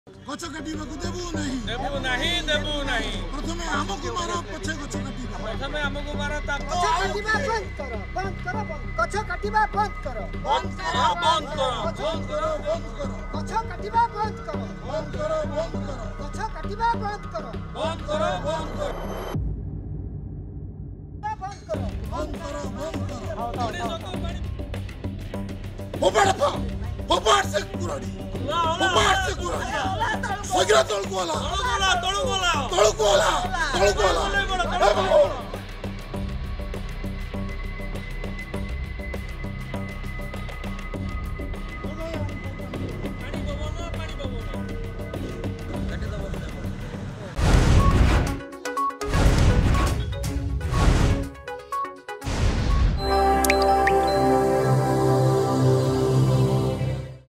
Protest Intensifies over felling of trees for MLA quarters in Bhubaneswar
Protest Intensifies by Mahanadi Bachao Andolan over felling of trees for MLA quarters in Bhubaneswar.